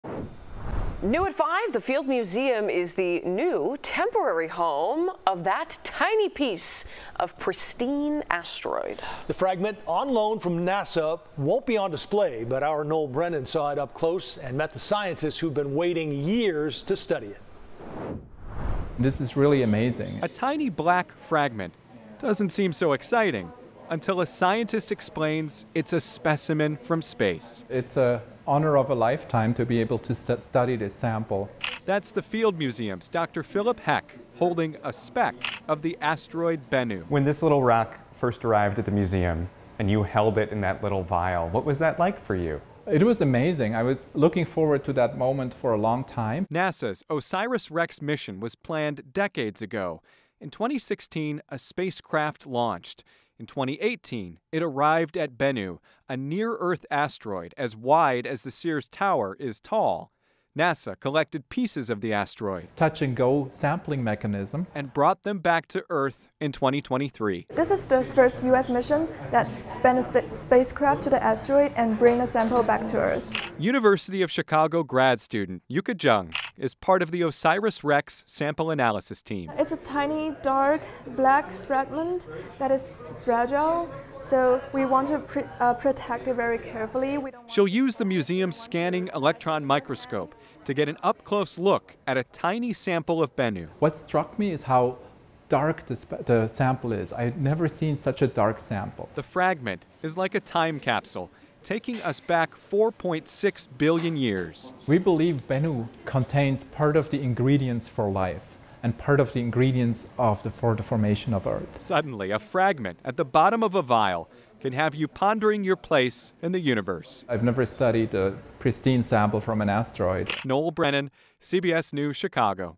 Unique asteroid sample lands at the Field Museum
The Field Museum is the new temporary home to a tiny piece of pristine asteroid. The fragment of the asteroid Bennu, on loan from NASA, won't be on display for visitors, but will give scientists the chance to study an asteroid sample uncontaminated by Earth's atmosphere.